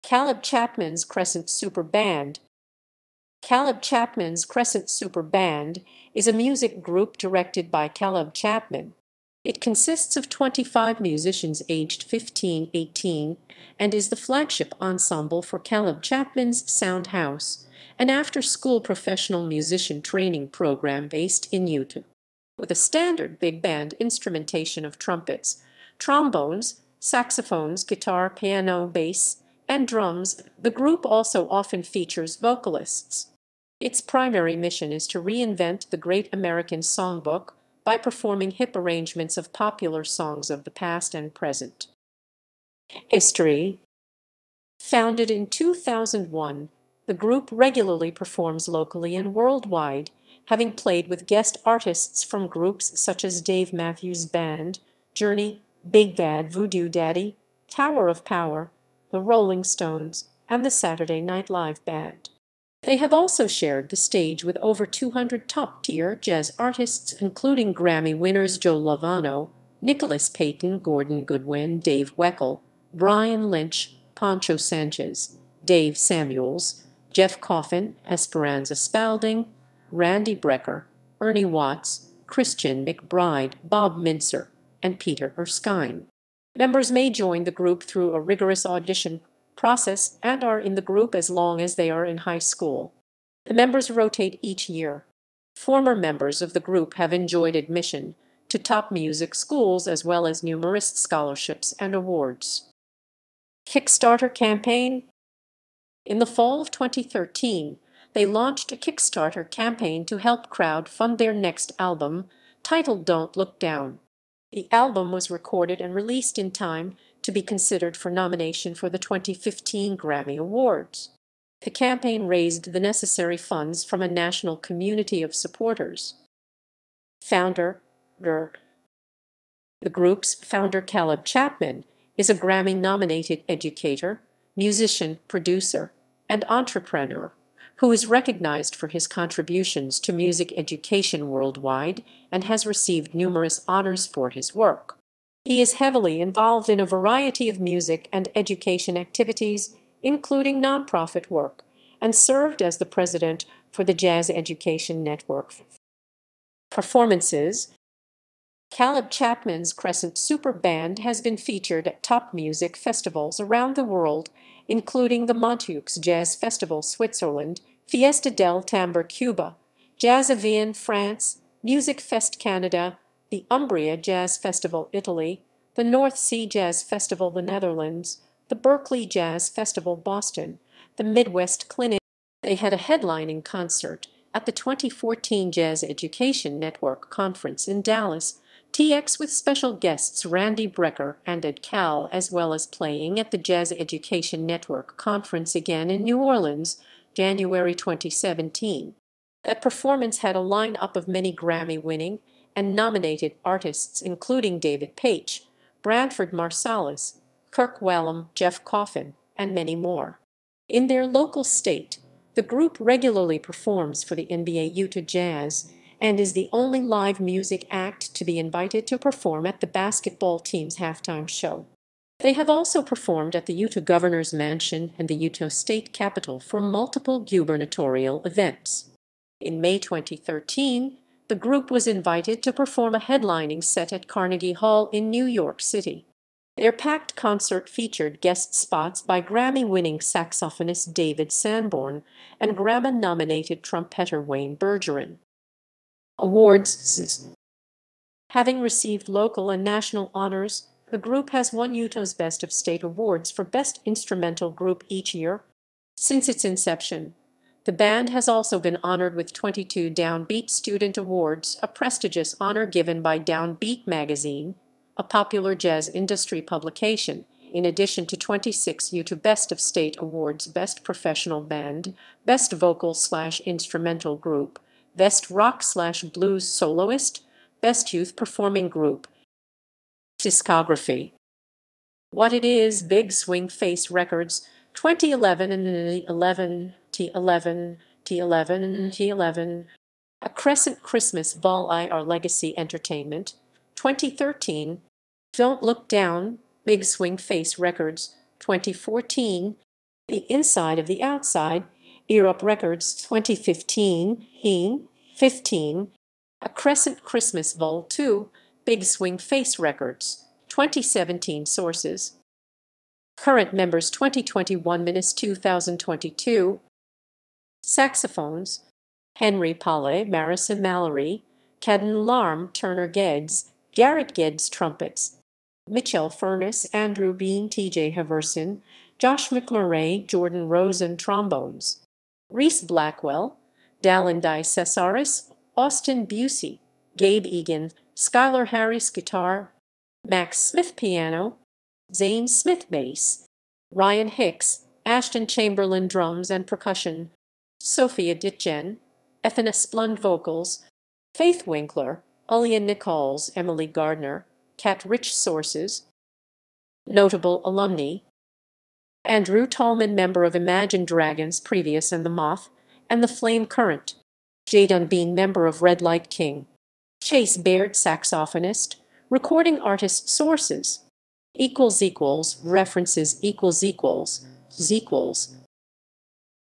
Caleb Chapman's Crescent Super Band
Caleb Chapman's Crescent Super Band is a music group directed by Caleb Chapman. It consists of 25 musicians aged 15–18 and is the flagship ensemble for Caleb Chapman's Soundhouse, an after school professional musician training program based in Utah.With a standard big band instrumentation of trumpets, trombones, saxophones, guitar, piano, bass, and drums, the group also often features vocalists. Its primary mission is to reinvent the Great American Songbook by performing hip arrangements of popular songs of the past and present.